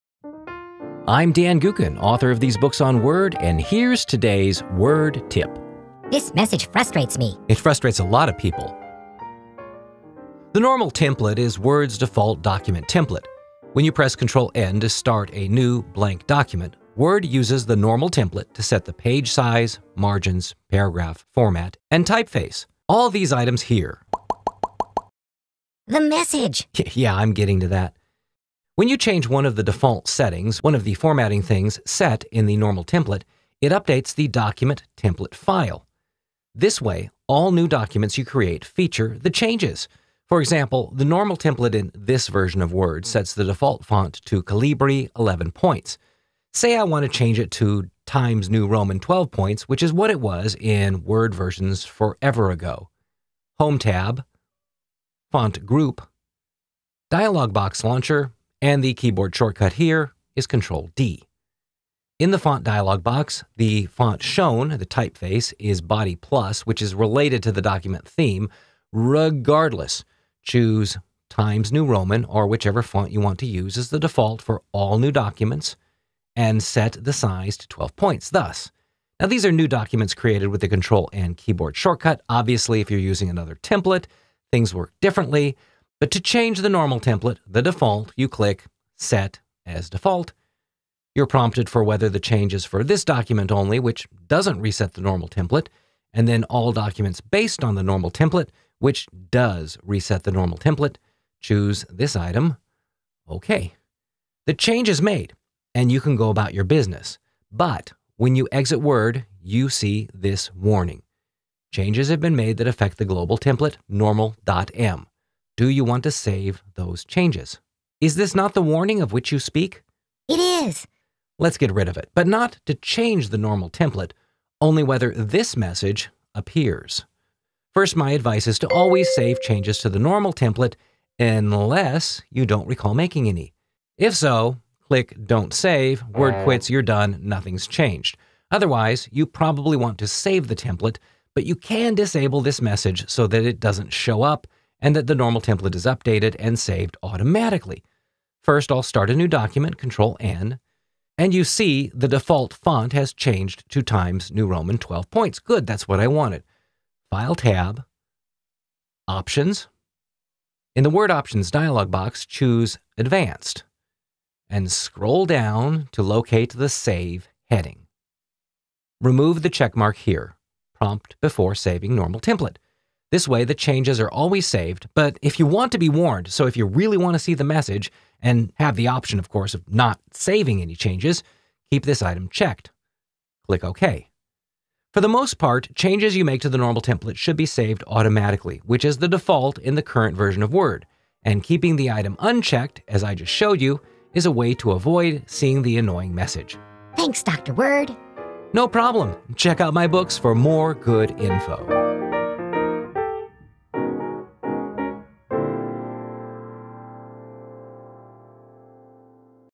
247 Saving the Normal Template
Word's default document template is named Normal. Changes to this template affect all new documents you create, which can be beneficial, though the message that appears may not make sense. Why not get rid of it?